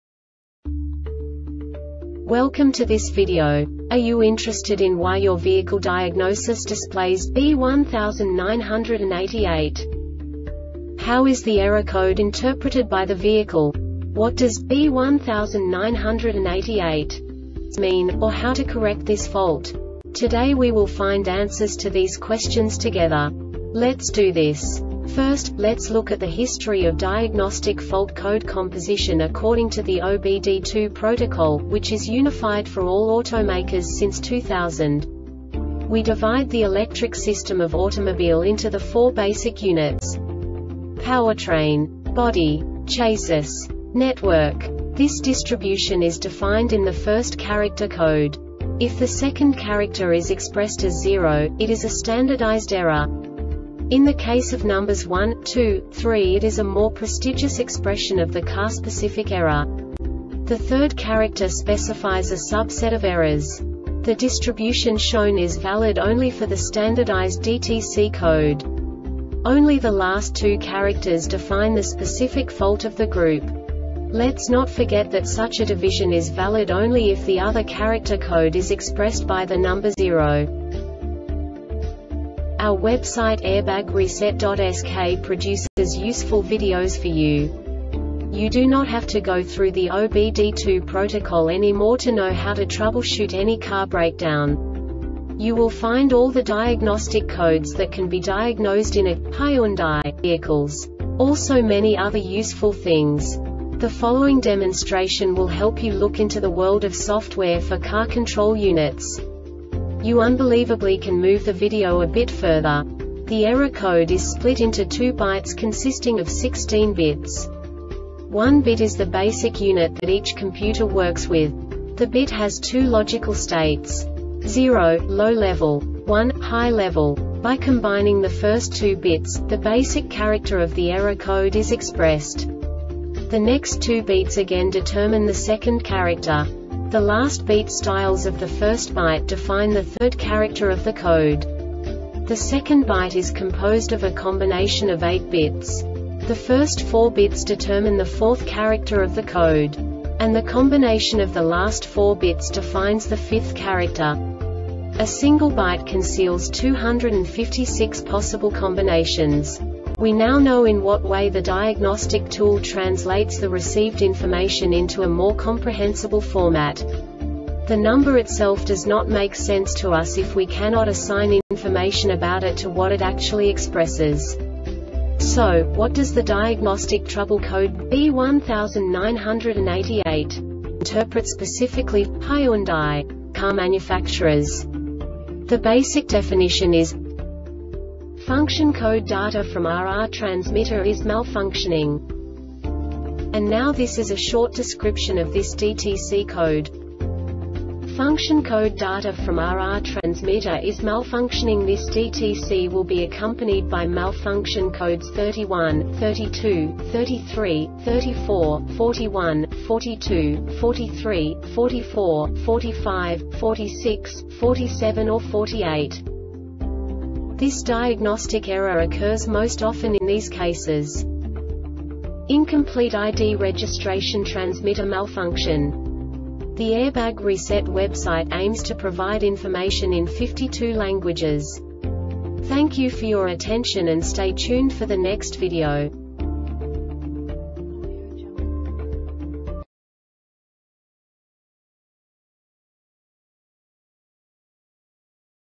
DTC Hyundai B1988 Short Explanation
Contents:
0:21 Basic DTC analysis according to OBD2 protocol standard.
1:48 Insight into programming
2:58 A diagnostically understandable description of the Diagnostic Trouble Code
3:05 Basic error definition

Make:
Hyundai
Code:
B1988
Definition:
Function Code Data from RR Transmitter is Malfunctioning.
Description:
Function code data from RR transmitter is malfunctioning.This DTC will be accompanied by Malfunction Code(s): 31, 32, 33, 34, 41, 42, 43, 44, 45, 46, 47 or 48
Cause: